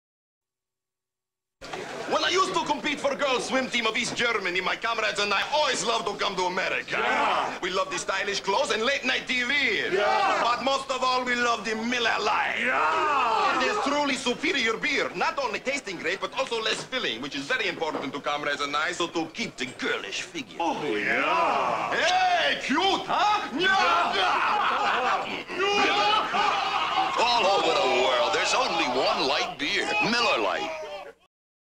Miller Lite commercial - Joe "Helga" Piscopo Ex-East German Swimmer (1986)
I never get tired of these old Miller Lite commercials.